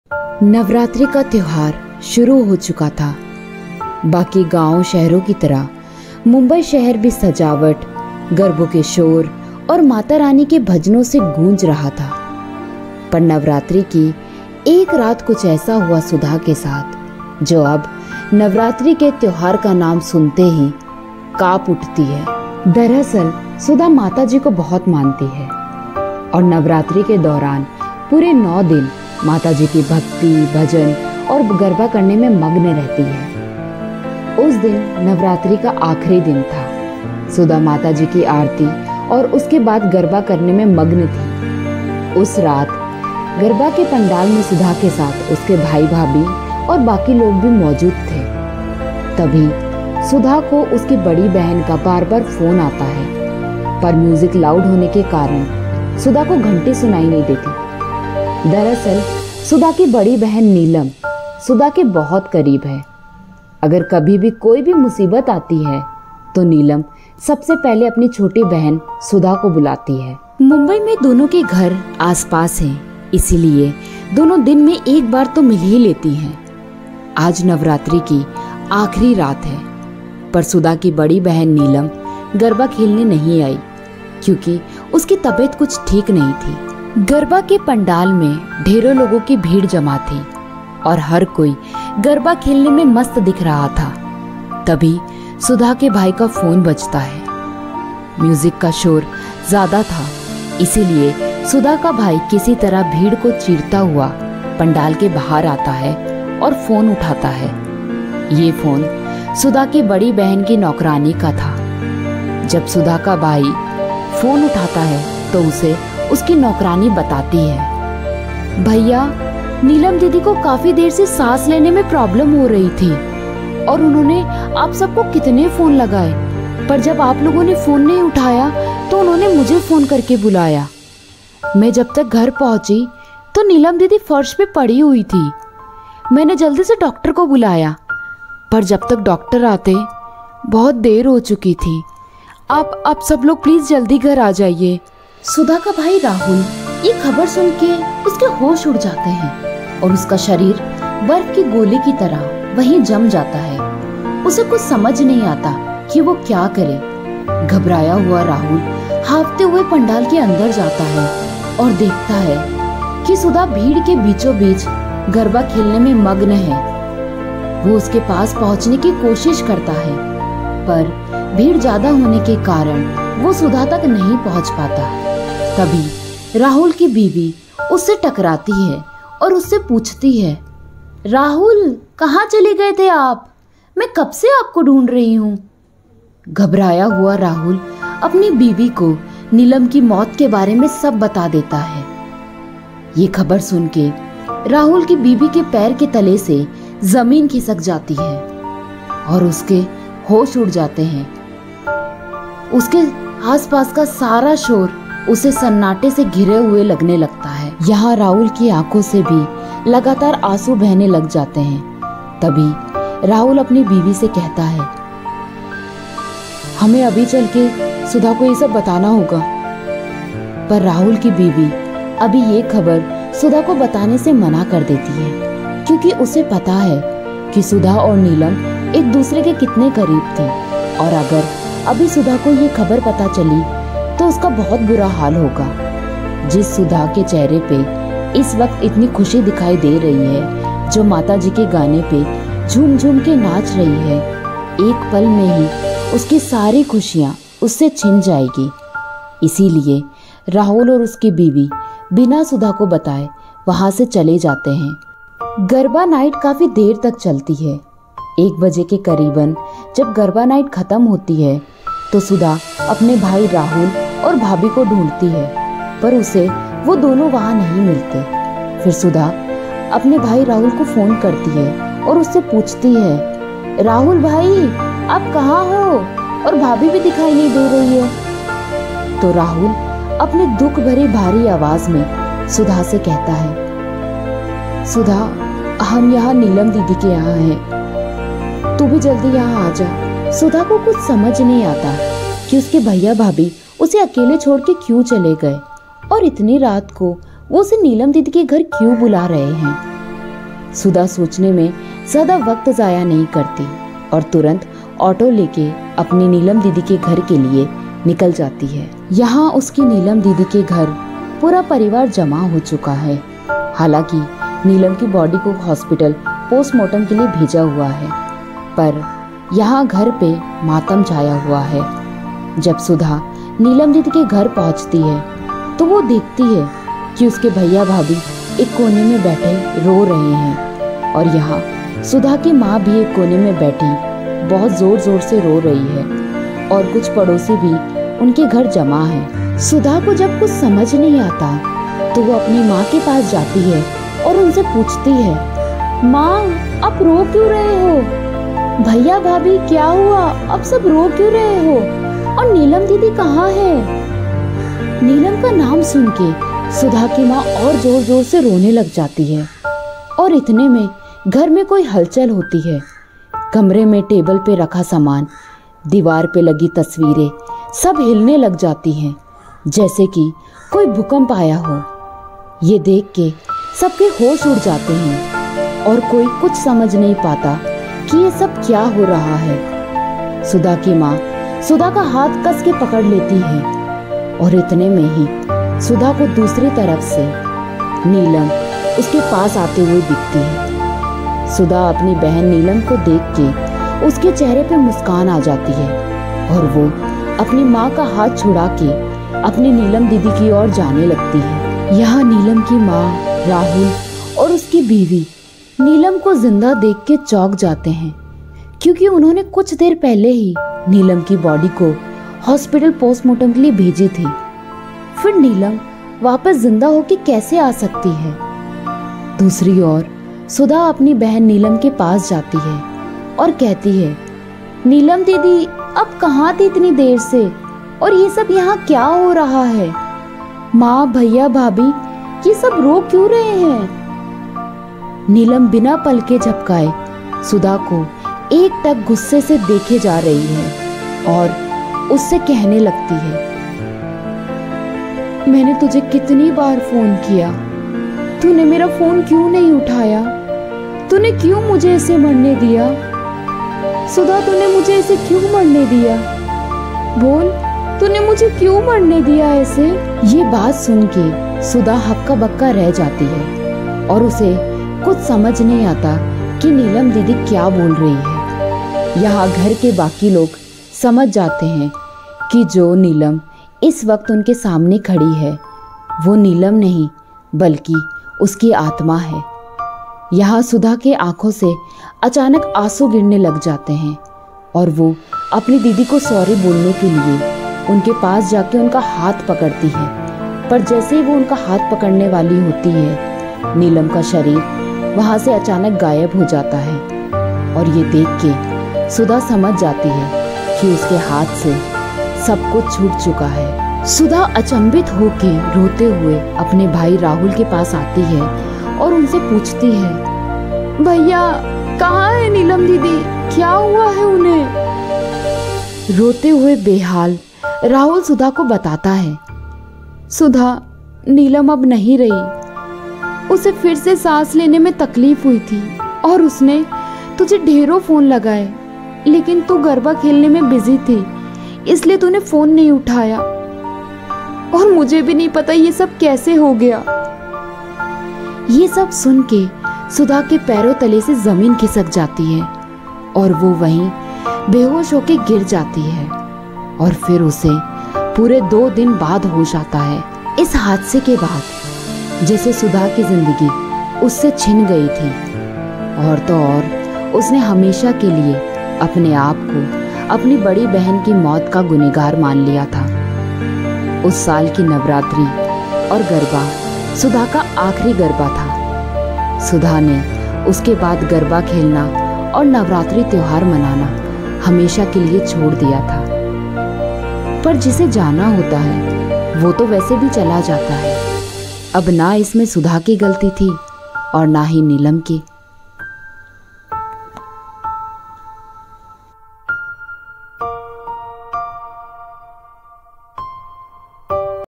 Navratri | Horror Real Story | By Pooja Jangid
Navrati
-Real Story

Sudha's sister Neelam died accidentally and her soul comes back to just tell Sudha that she called her when she was taking her last breaths and she could save her but she didn't.